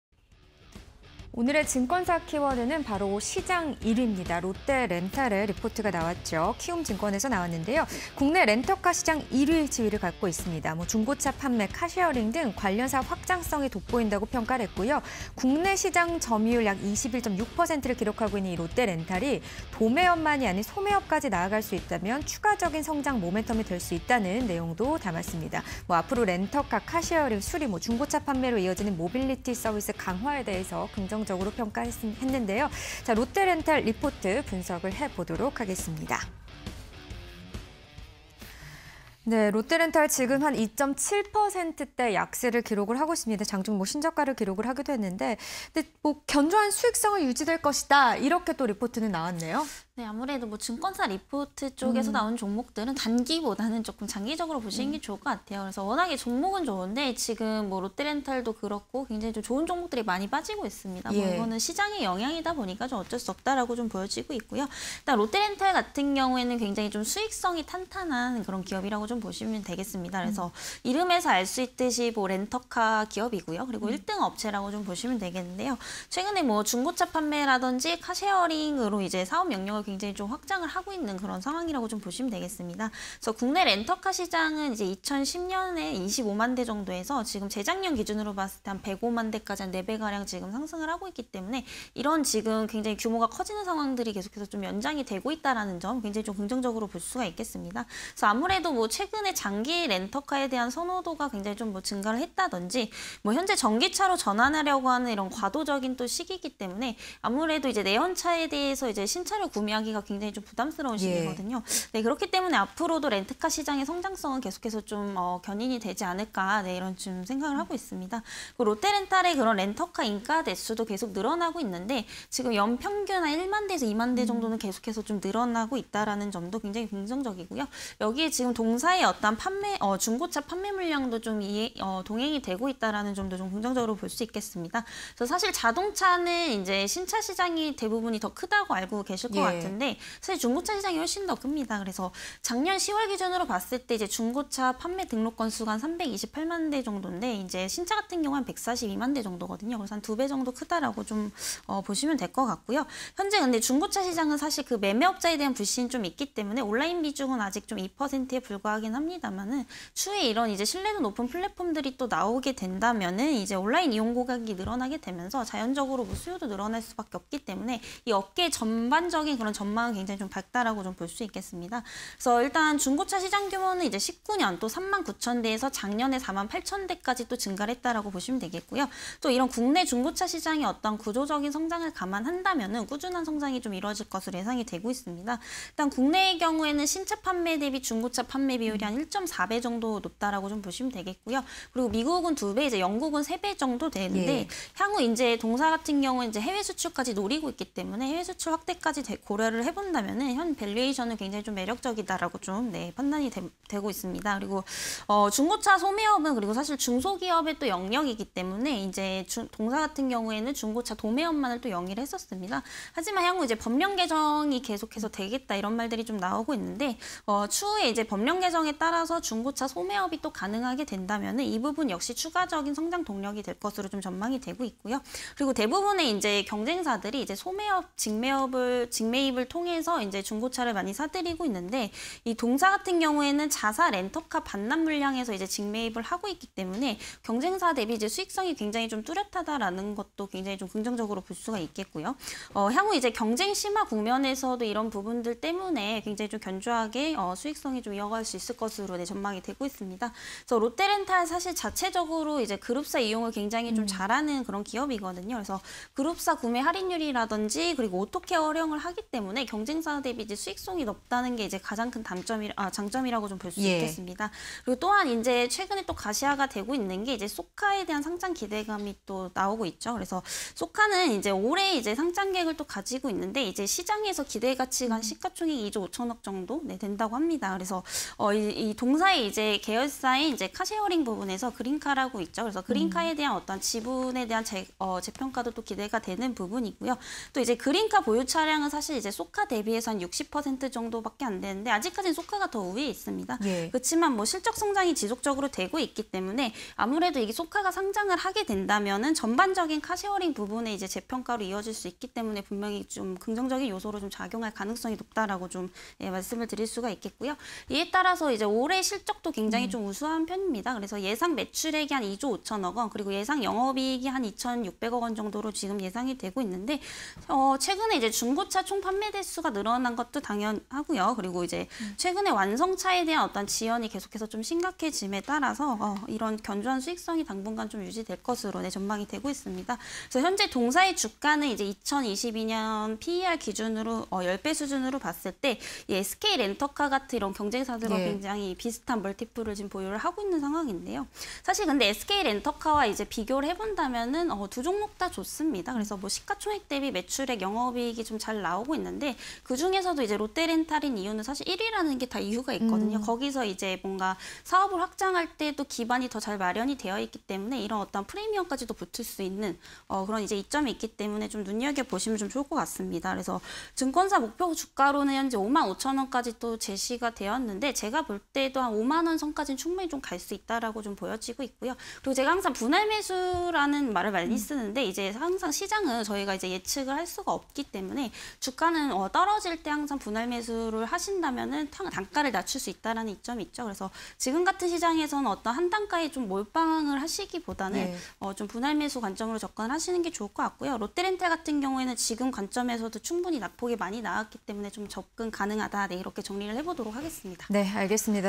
롯데렌탈(089860), 국내 렌터카 시장 1위… 사업 확장성↑_키워드 팩토리 (20220125)
롯데렌탈(089860), 국내 렌터카 시장 1위… 사업 확장성↑
- 국내 렌터카 시장 성장세
- 국내 시장 1위·글로벌 6위 규모 사업자
- 중고차 판매 법령 개정… 성장 동력 예상
- 자회사 '그린카', 성장 견조
- 경쟁사 '쏘카' 상장… '그린카' 지분가치 재평가 기대
- 올해 호실적 전망